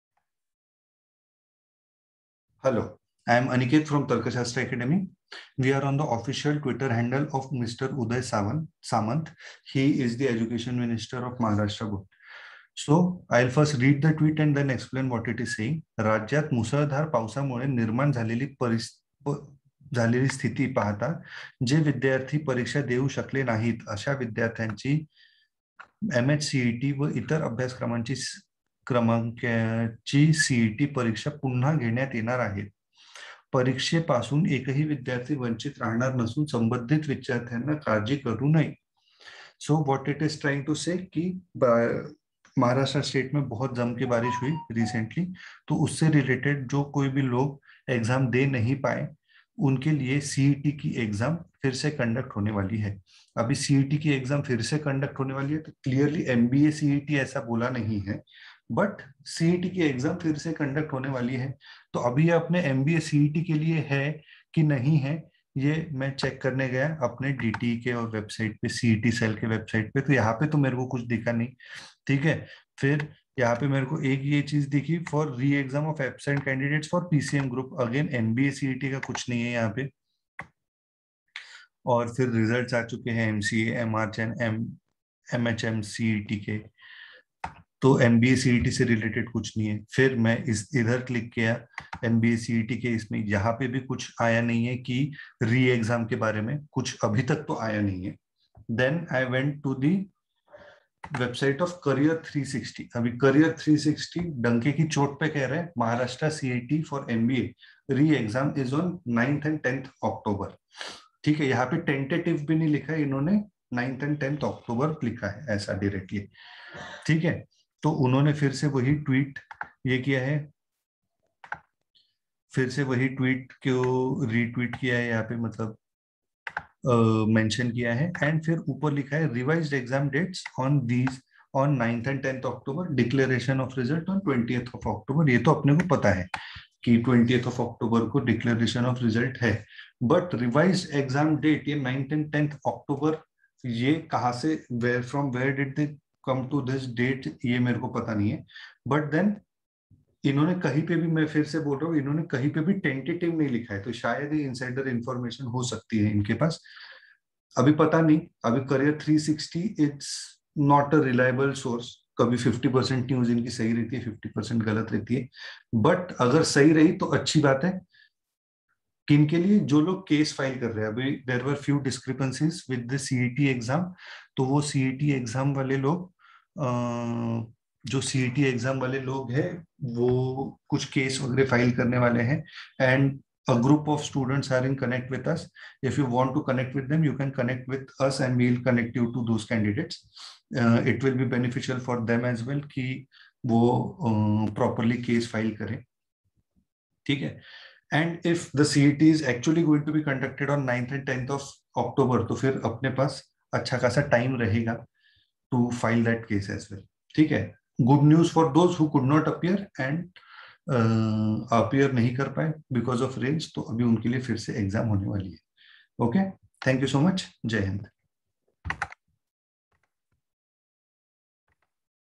CET 2021 Re-Exam? | Tweet by Mr. Uday Samant | News from Unofficial source | Result May be Delayed
CET 2021 Re-Exam? | Mr. Uday Samants Tweet | News from Unreliable source